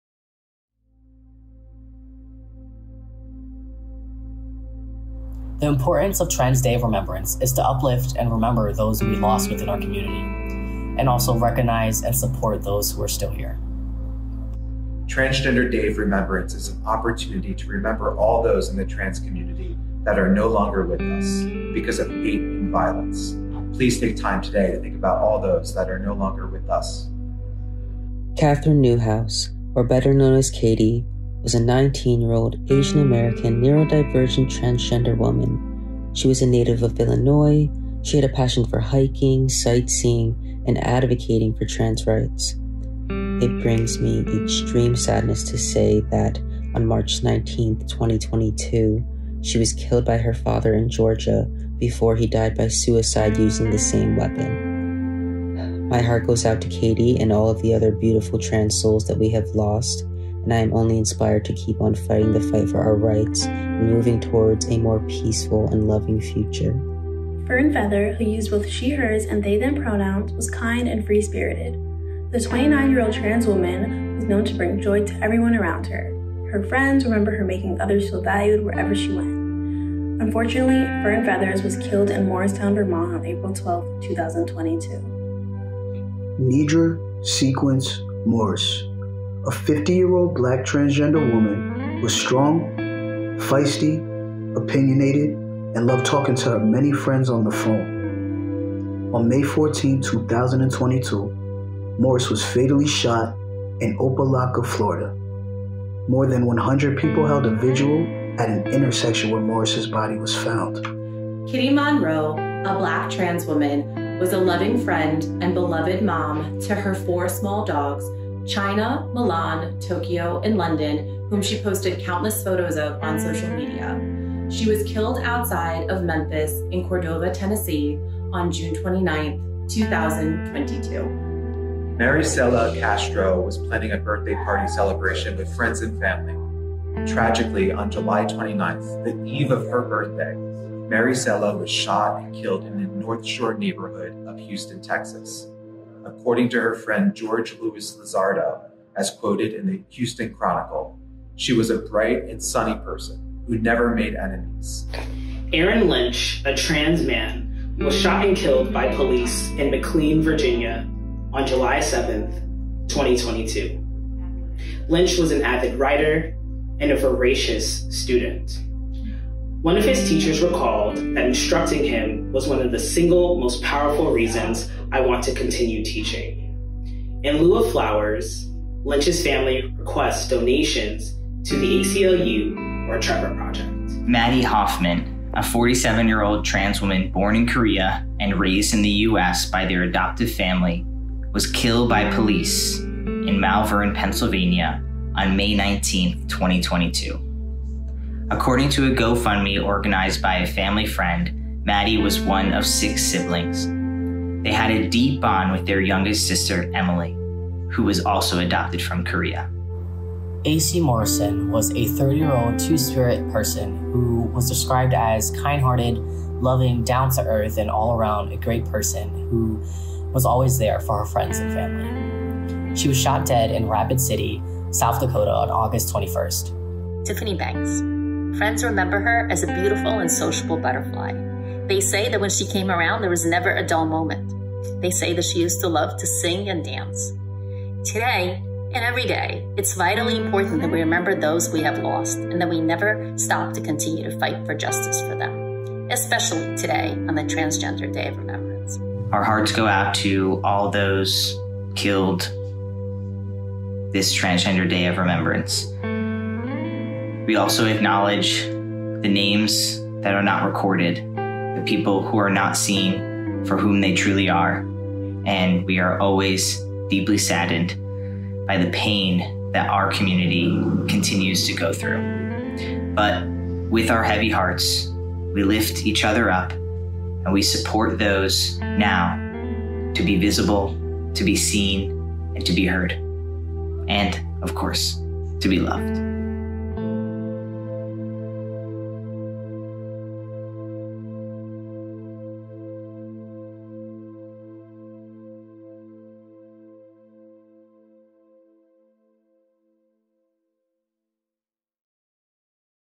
Transgender Day of Remembrance 2022
At least 32 transgender people have been killed in the US so far in 2022. That doesn't include those whose stories went unreported or misreported. In this video, we memorialize just some of those who were killed this year. Please take some time today to honor the memories of these people.

Today, we are also holding in our thoughts those who have been impacted by the mass shooting at Club Q, an LGBTQ+ nightclub, in Colorado Springs. This senseless violence would have been tragic no matter when it happened, but for it to come on the eve of Transgender Day of Remembrance makes it even more gut-wrenching.

For more information on those we've lost this year, please